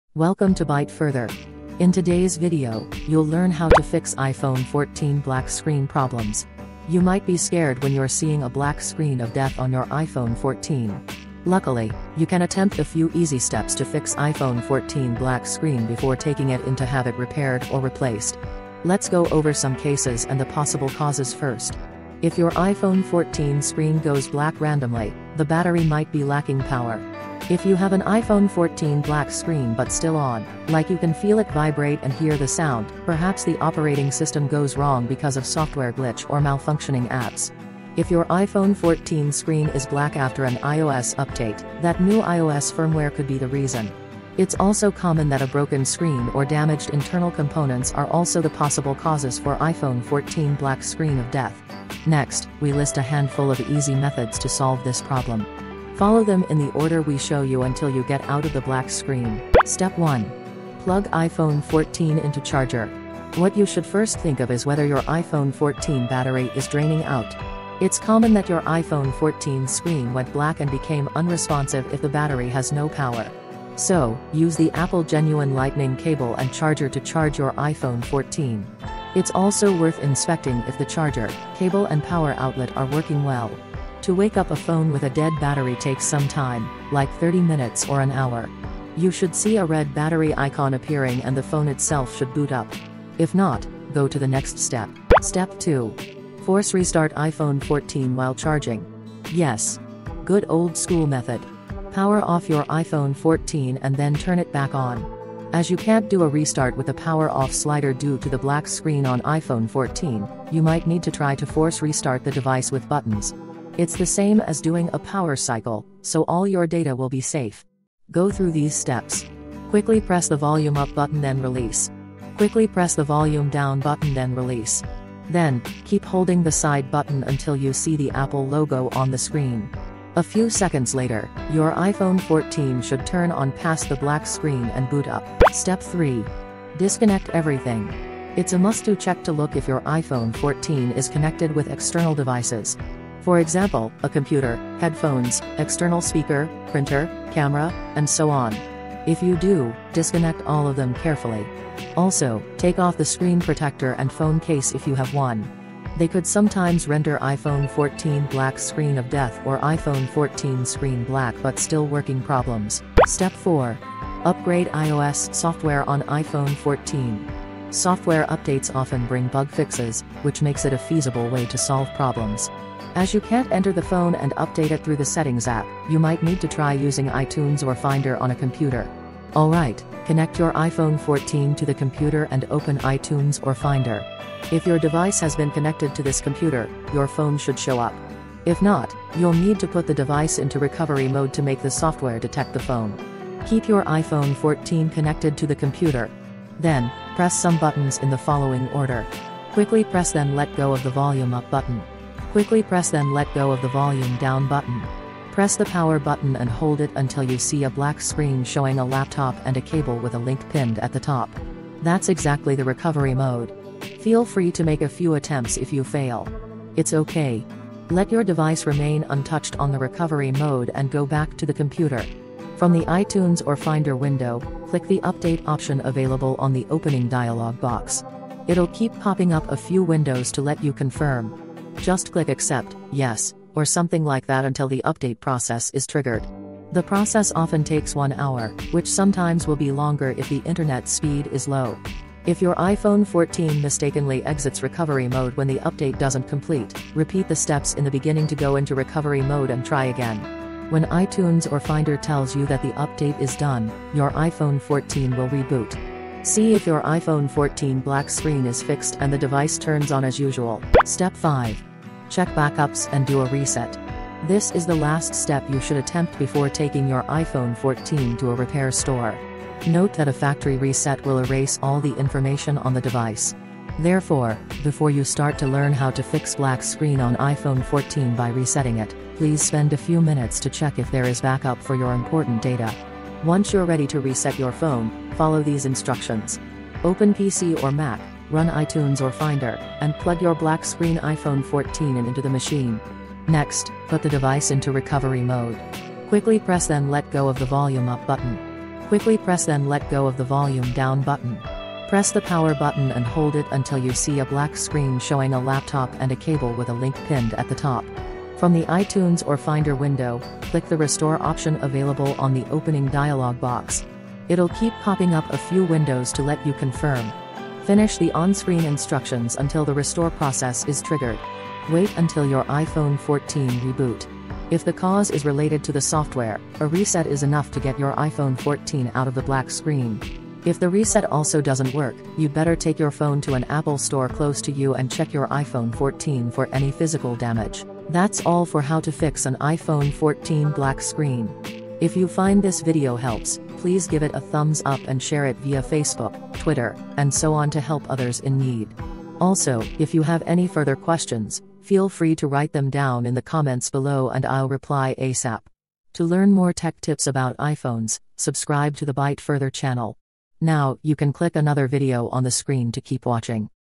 iPhone 14 Screen Goes Black Randomly? Try These 5 Steps to Fix Black Screen
In today’s video, you’ll learn how to fix iPhone 14 black screen problems. You might be scared when you’re seeing a black screen of death on your iPhone 14. Luckily, you can attempt a few easy steps to fix iPhone 14 black screen before taking it in to have it repaired or replaced.

Let's go over some cases and the possible causes first.
If your iPhone 14 screen goes black randomly, the battery might be lacking power.
If you have an iPhone 14 black screen but still on, like you can feel it vibrate and hear the sound, perhaps the operating system goes wrong because of software glitch or malfunctioning apps.
If your iPhone 14 screen is black after an iOS update, that new iOS firmware could be the reason.
It’s also common that a broken screen or damaged internal components are also the possible causes for iPhone 14 black screen of death.

Next, we list a handful of easy methods to solve this problem. Follow them in the order we show you until you get out of the black screen.

0:59 Step One. Plug iPhone 14 into charger
1:35 Step Two. Force Restart iPhone 14 while charging
2:15 Step Three. Disconnect everything
2:42 Step Four. Upgrade iOS software on iPhone 14
4:20 Step Five. Check backups and do a reset

Hope this video can help you fix iPhone 14 black screen problems.